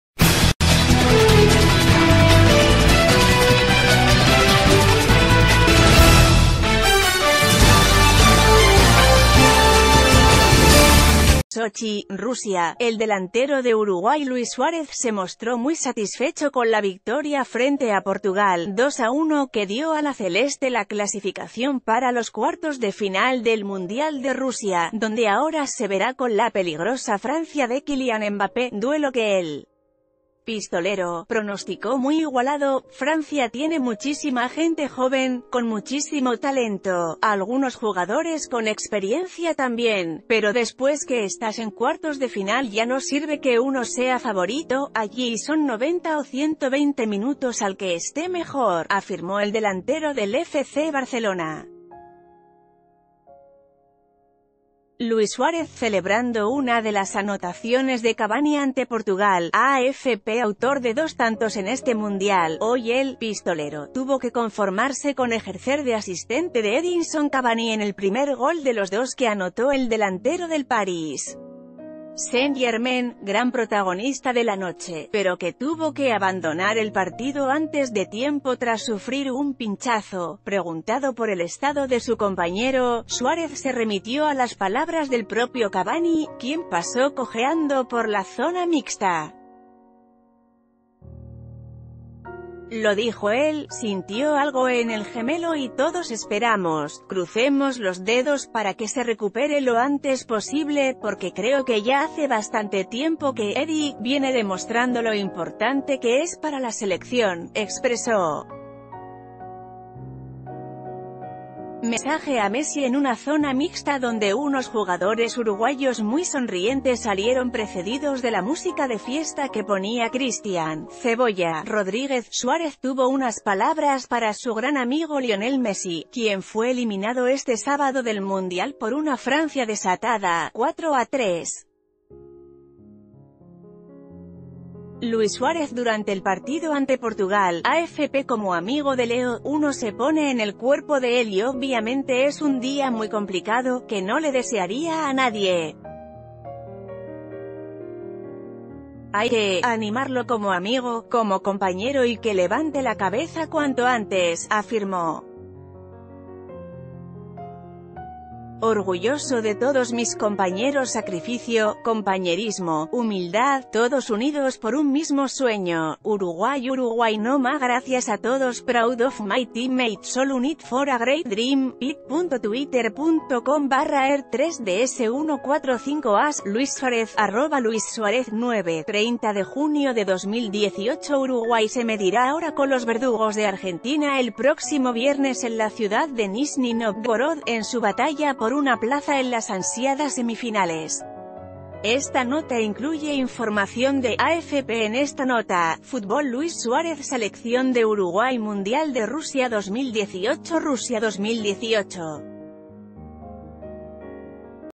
Suárez no le teme a Francia "En cuartos ya no sirve que uno sea favorito"
Sochi, Rusia.- El delantero de Uruguay Luis Suárez se mostró muy satisfecho con la victoria frente a Portugal (2-1) que dio a la Celeste la clasificación para los cuartos de final del Mundial de Rusia, donde ahora se verá con la peligrosa Francia de Kylian Mbappé, duelo que el 'Pistolero' pronosticó...